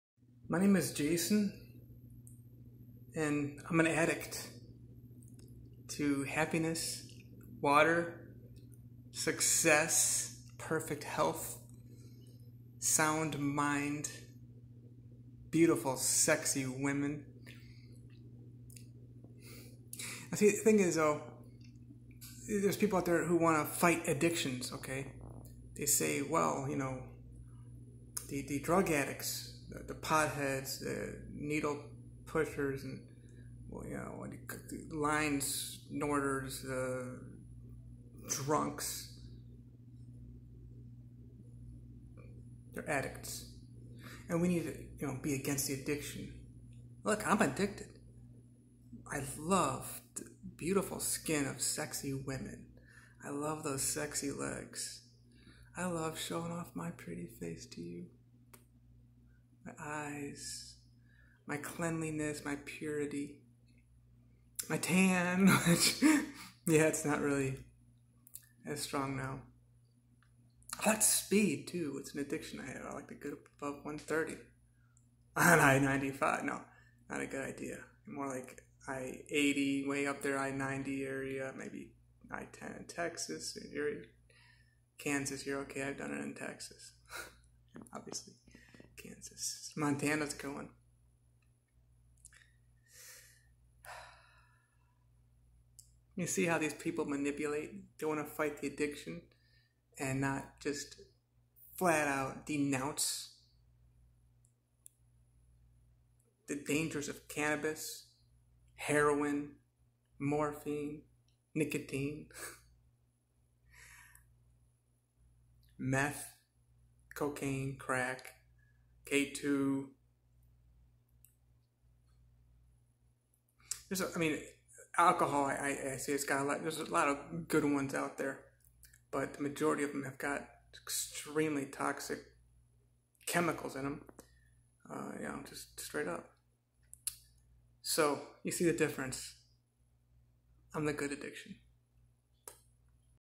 I am an addict
so I am admitting my addictions now coming out to tell you the truth about my drugs of choice